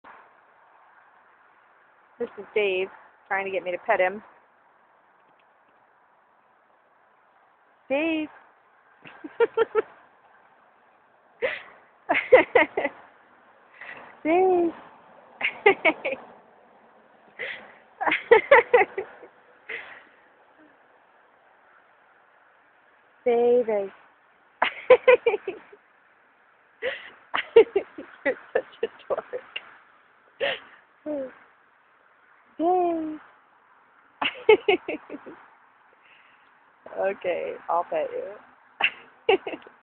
Dave
Here is Dave, squirming around, trying to get me to pet him when I've gotten home from work.  He's such a dork!  He's been running around in the garden so his feet are all muddy.  lol